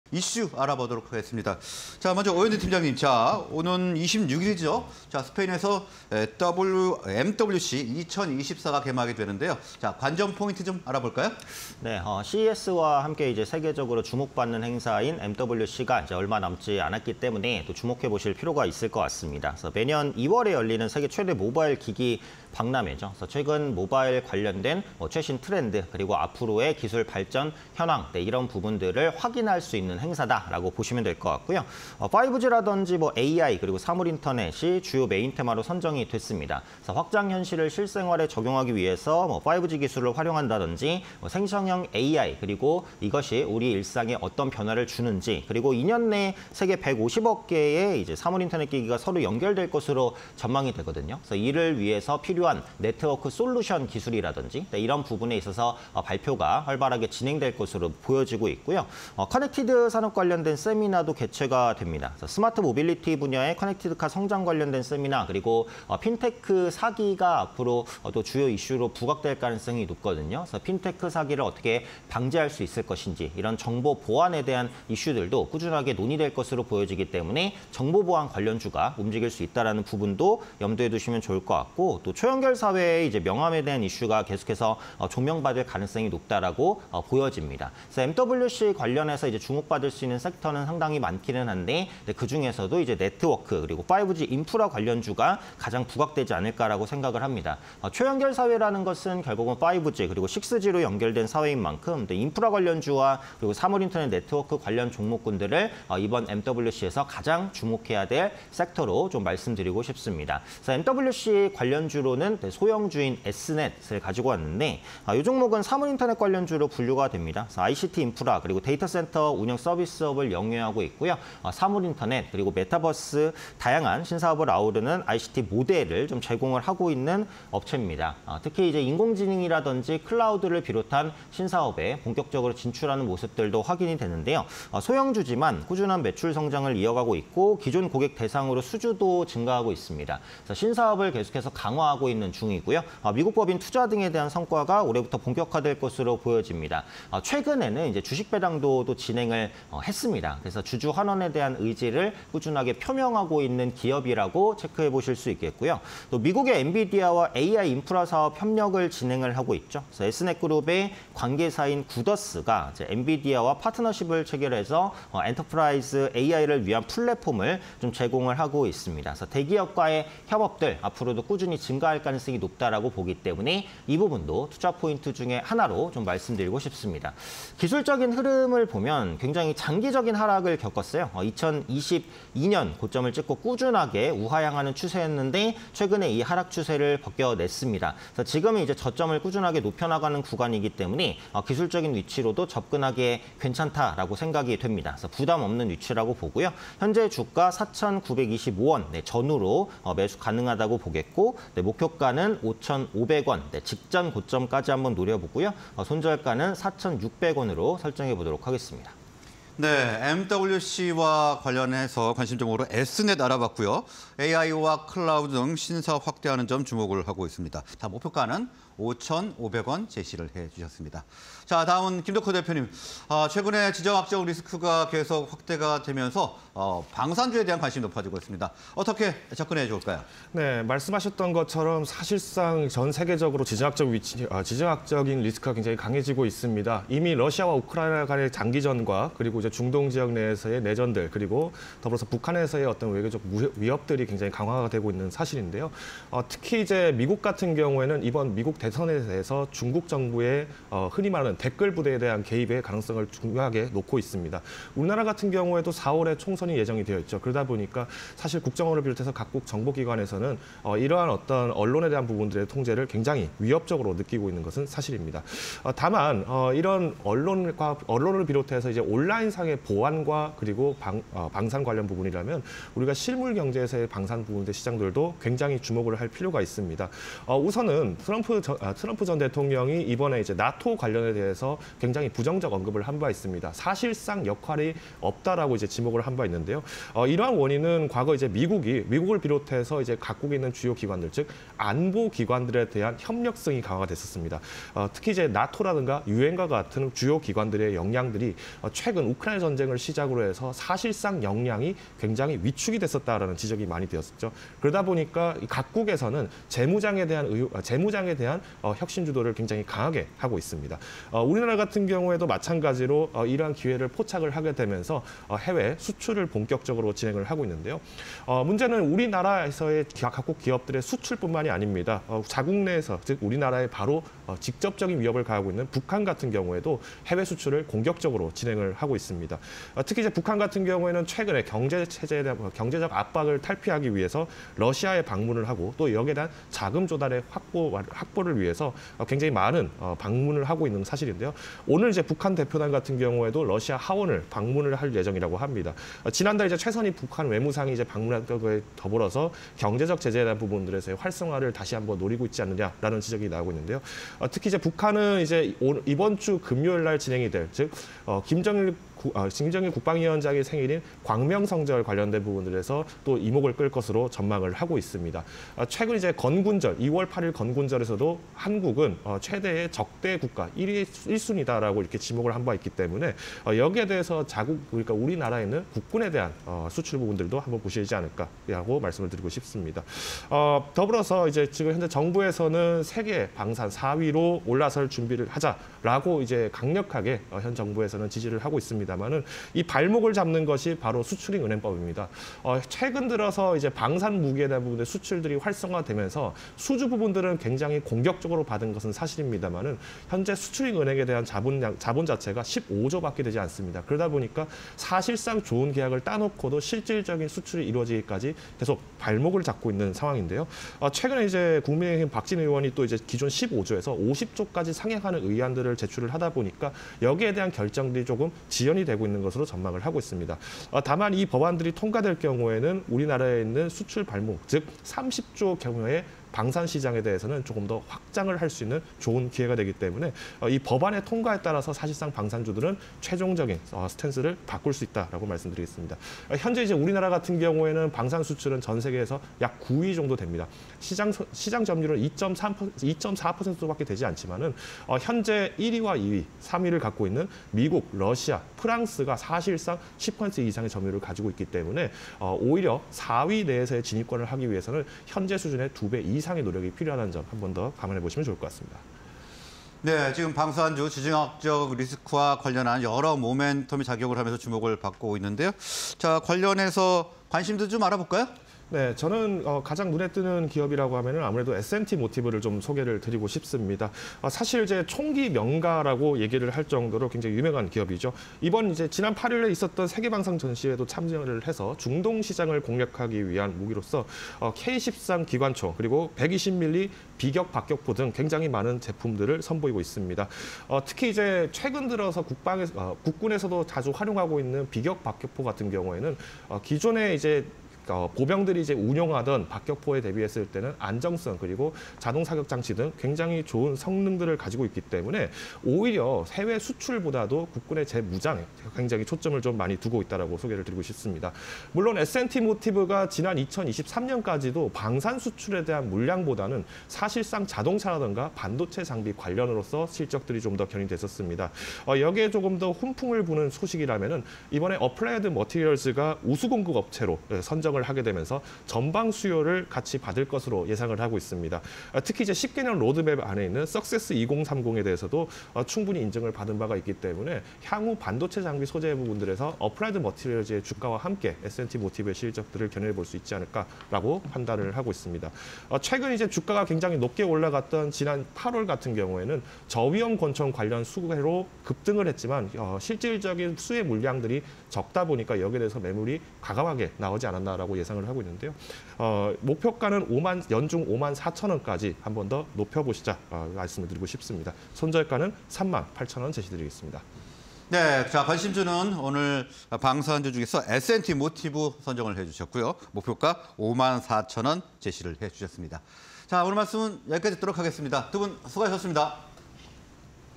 에스넷(038680)... 인공지능·클라우드 등 신사업 확대 / SNT모티브(064960)... 세계방산전시회 참전... 중동 시장 공략_개장 관심주 (20240213)
Q. 다가오는 'MWC 2024'... 관전포인트는?
Q. 진격의 K-방산, 4대 수출국으로 거듭날까?

에스넷(038680)
- ICT인프라·데이터 센터 운영서비스업 영위
- 인공지능·클라우드 등 신사업 확대
- 美 엔비디아와 AI인프라 사업 협력

SNT모티브(064960)
- 세계방산전시회 참전... 중동 시장 공략
- AMAT, 우수 공급업체 선정... 전방수요 증가 수혜